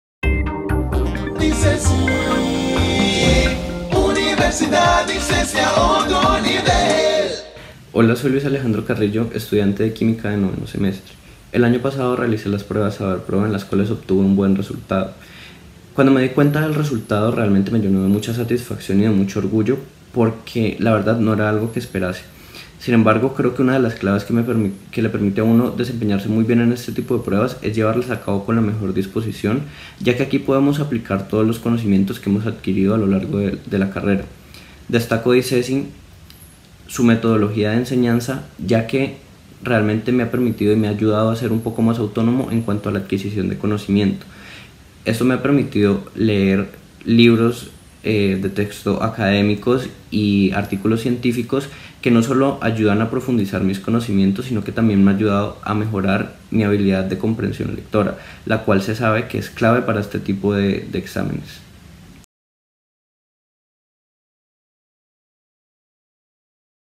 SeValeEstarFelices
MejorSaberPro Química "La metodología de enseñanza de Icesi me ha permitido ser más autónomo en la adquisición de conocimiento, de esta manera he mejorado la comprensión lectora, uno de los aspectos claves en este tipo de pruebas", Luis Alejandro Carrillo, estudiante de noveno semestre del programa de Química, mejor Saber Pro 2019. ¡Felicitaciones!